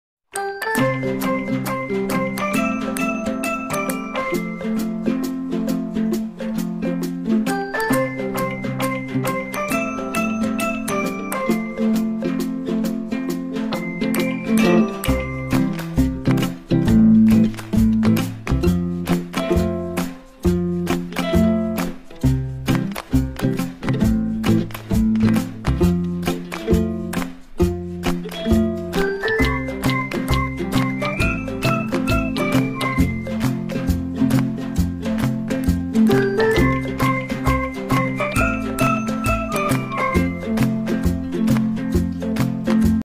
DIY Golden leaf Making, April 23, 2020
Best out of waste with cardboard and kitchen foil